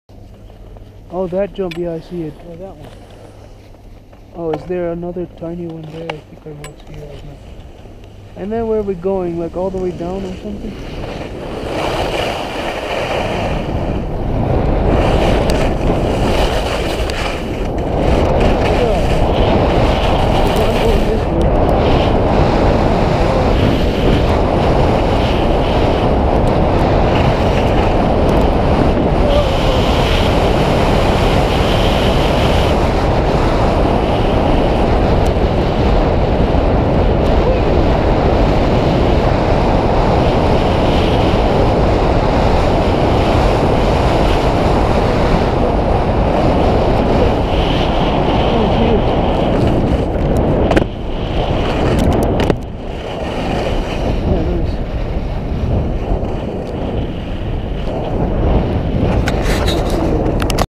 short fun red run in Grand bornand
red run with nobody on the slopes and plenty of airtime spots. There is also an easy but fun off piste area next to it that I will do when the snow is better
I beleive this is a blue on the map and I'm not sure which one it is.